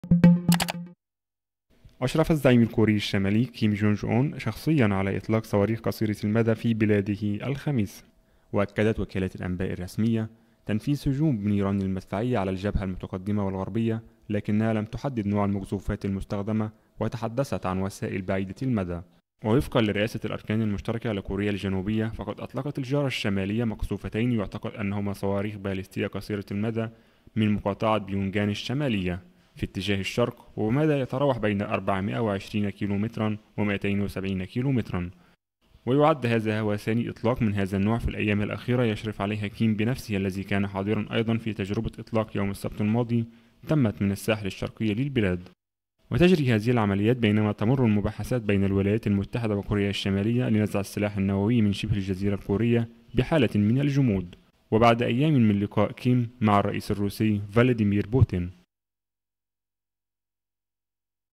كيم جونج أون أشرف بنفسه على إطلاق الصواريخ الأخيرة في كوريا الشمالية
أشرف الزعيم الكوري الشمالي، كيم جونج أون، شخصيا على إطلاق صواريخ قصيرة المدى في بلاده الخميس، وفقا لما ذكرته وكالة أنباء (KNCA) الكورية الشمالية الجمعة.